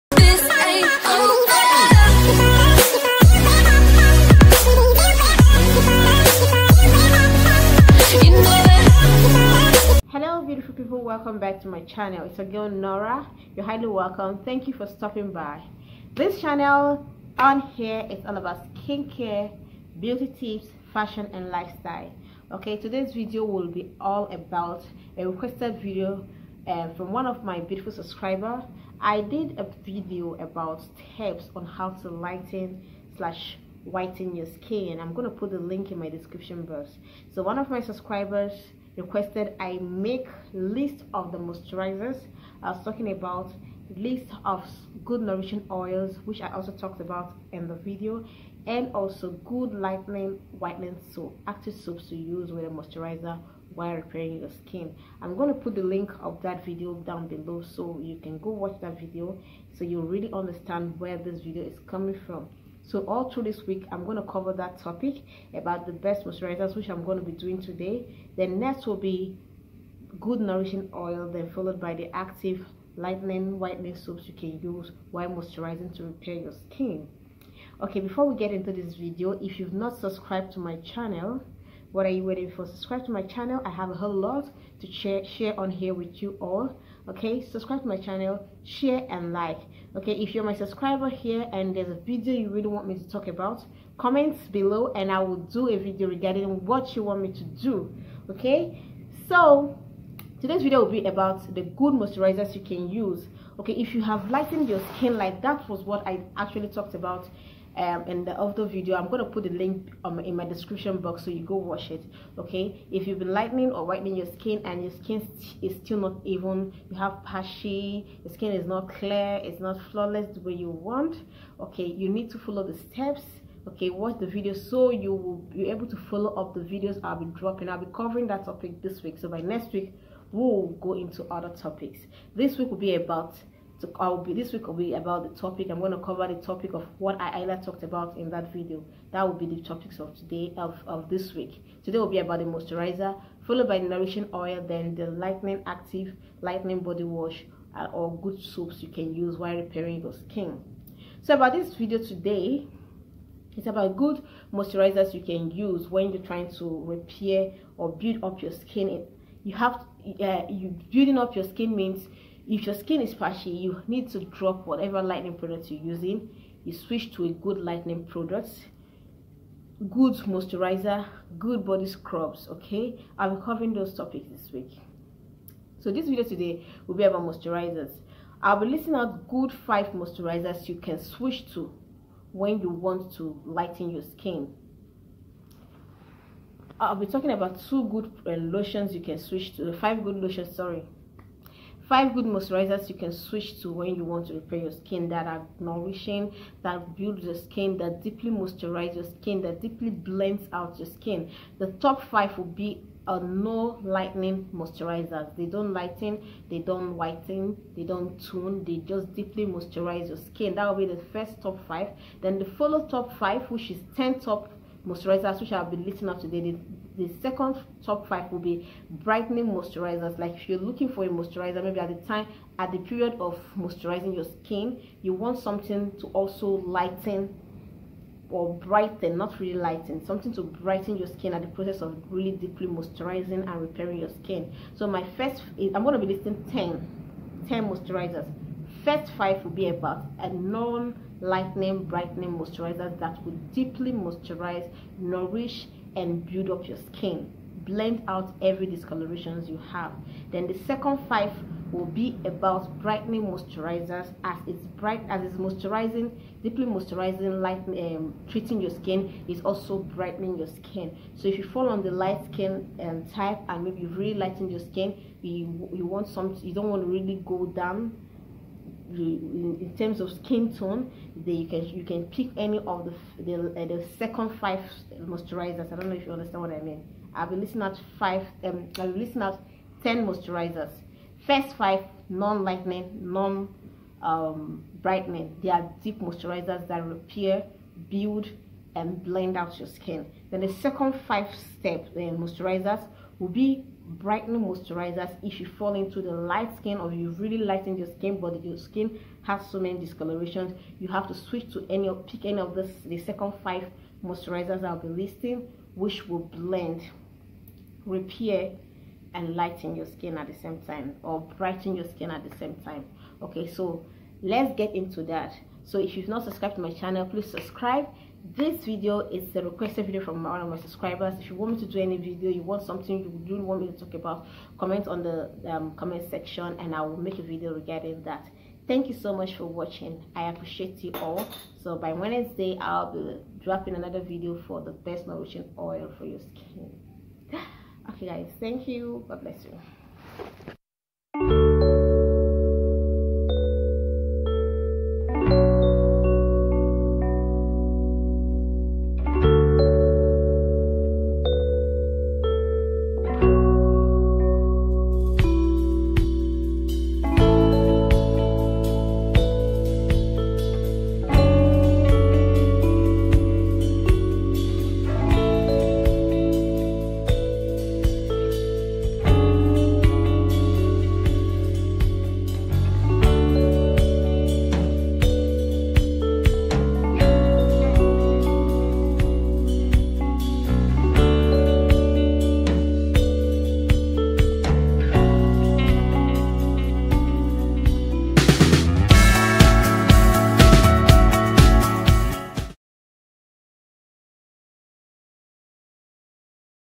Moisturiser Top 10 Best moisturisering skin repair lotions
Today's video is all about, Best moisturisering, skin repair lotions, to blends out dark patchy skin and discoloration , to help bring skin back to its normal healthy way

Comment at d comment section the video u will want to see next, and I will make a video regarding dat